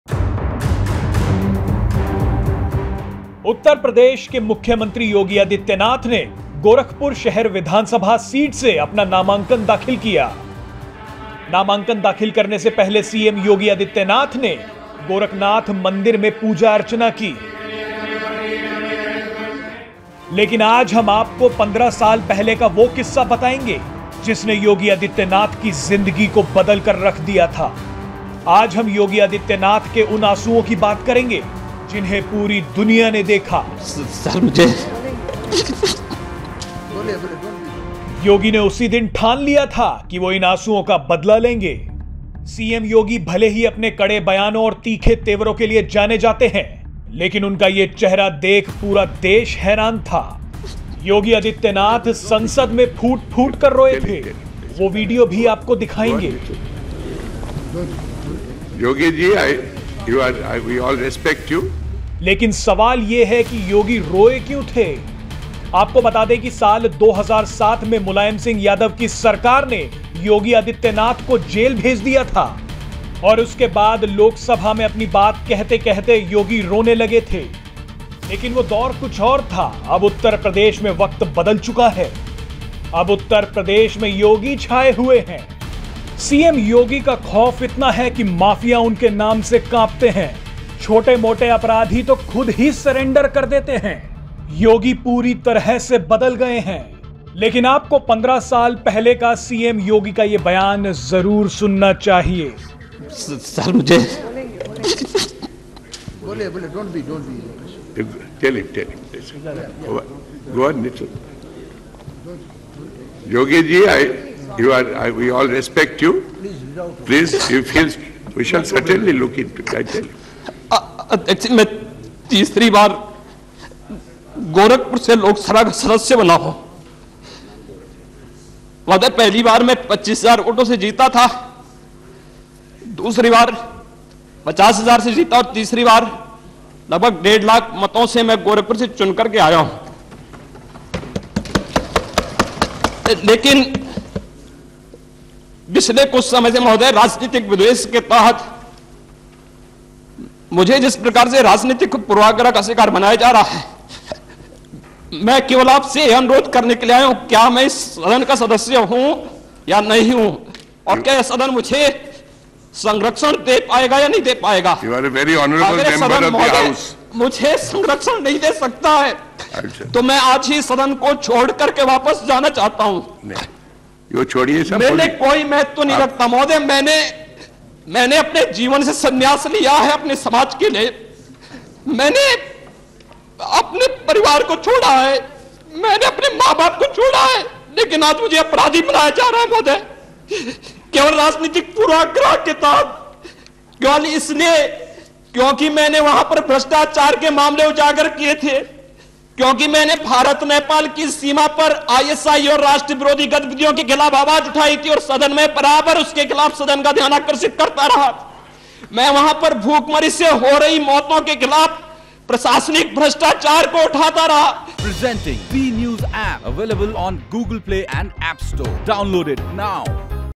CM Yogi Oath Ceremony: 2 मिनट तक रोते रहे योगी, मांगी थी मदद, आज देखिए ताकत |Yogi Adityanath | Live
Yogi Adityanath will take oath as the Chief Minister of Uttar Pradesh on Friday (March 25, 2022). The BJP leaders is all set to script history and take oath as the CM of Uttar Pradesh for a new term in office after completing a full five-year term, a feat none of his predecessors has been able to achieve.

UP Election Result 2022 Live Updates : 2 मिनट तक रोते रहे योगी, मांगी थी मदद, आज देखिए ताकत | CM Yogi Adityanath | UP Election 2022 |

यूपी के मुख्यमंत्री योगी आदित्यनाथ ने गोरखपुर शहर व‍िधानसभा सीट से अपना नामांकन दाखिल किया.. नामांकन दाख‍िल करने से पहले सीएम योगी ने गोरखनाथ मंदिर में पूजा-अर्चना की.. लेकिन आज हम आपको 15 साल पहले का वो किस्सा बताएंगे जिसने योगी आदित्यनाथ की ज़िन्दगी को बदलकर रख दिया था..